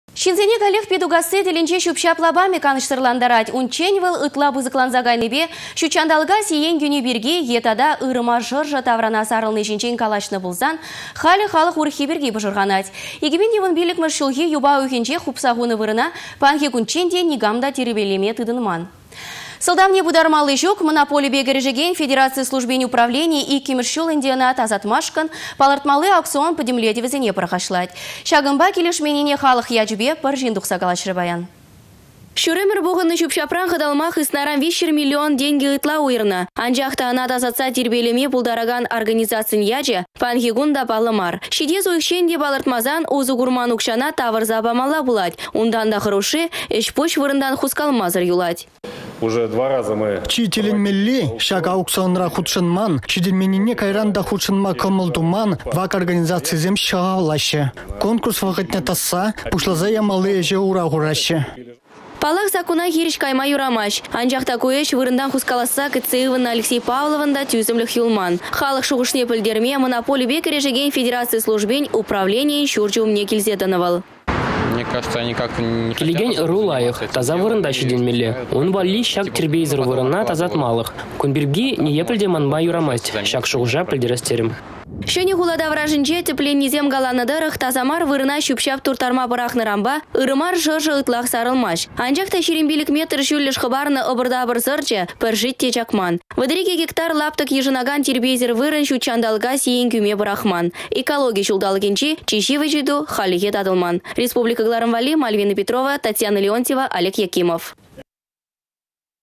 «Панă укçа - кирлĕ мар»
Сăлтавне пытармалли çук — монополипе кĕрешекен федераци службин управленийĕ 2-мĕш çул ĕнтĕ ăна тасатмашкăн палăртмалли аукцион пĕтĕмлетĕвĕсене пăрахăçлать. Çакăнпа килĕшменнине халăх ячĕпе 1 çын тухса калаçрĕ.